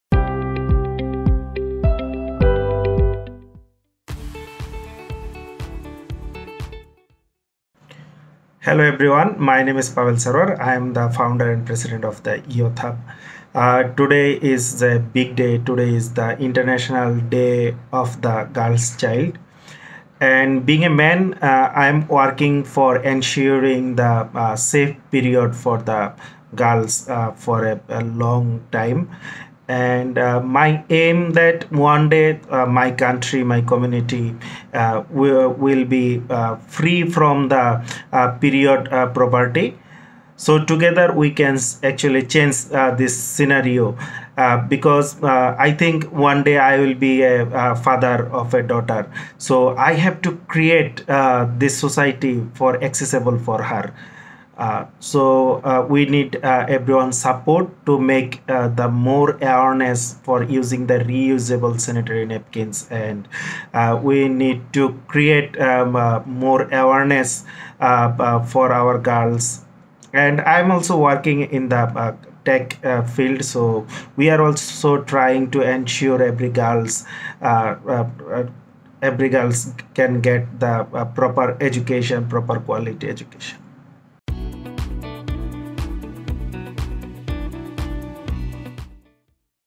Celebrating International Day of The Girl Child
In celebration of the International Day of The Girl Child, Pavel Sarwar, President of Youth Hub is sharing his thoughts on the importance of creating a period friendly Bangladesh for every girl.